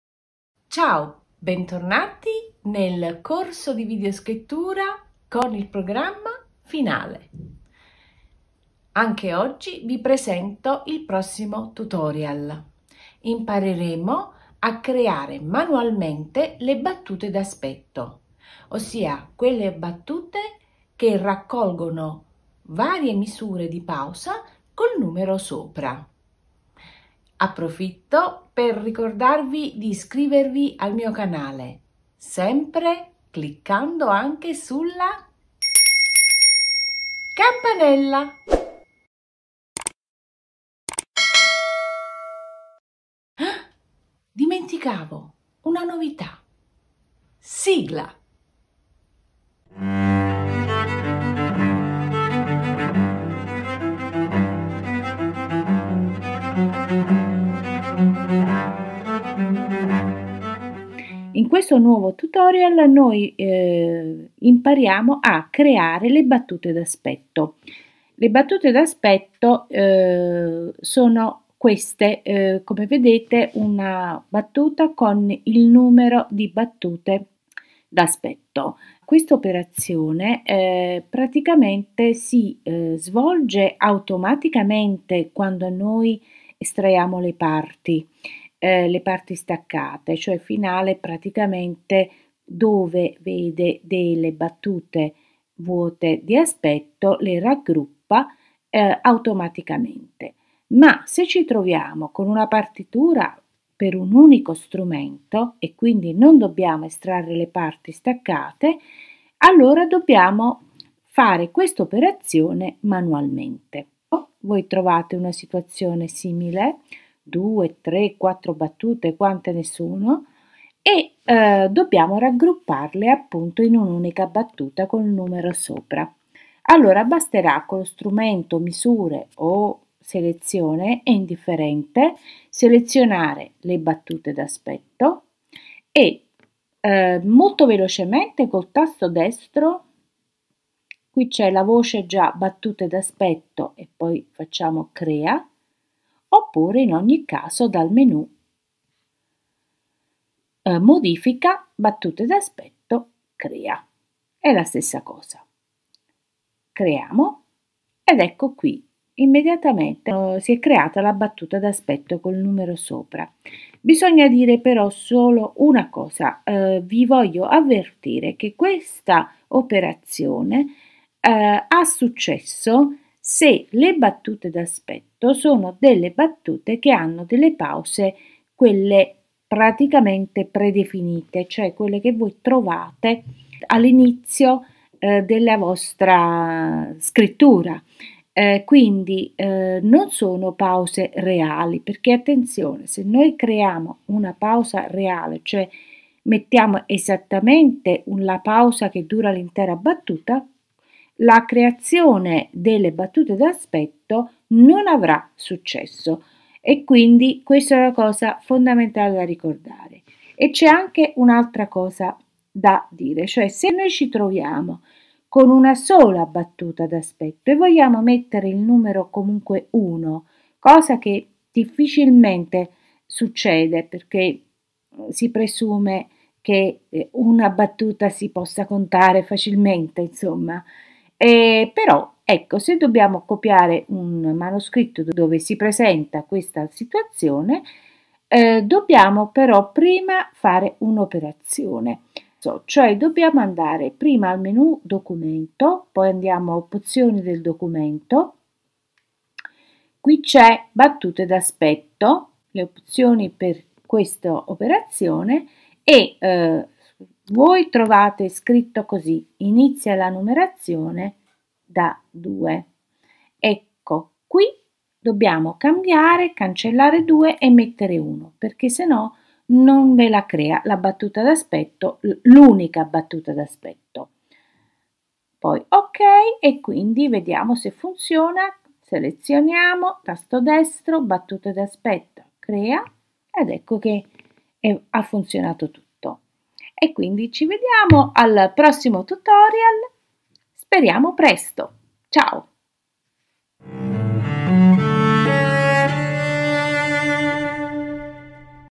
Susanna Canessa Finale Tutorial 23 Battute d'aspetto
Corso su Finale Software versione 2014 italiano
Tutorial 23: CREARE LE BATTUTE D'ASPETTO (manualmente)
(doc. di Informatica musicale e Videoscrittura al Conservatorio di Avellino).
L'uso di questo Software richiede la conoscenza della Teoria musicale.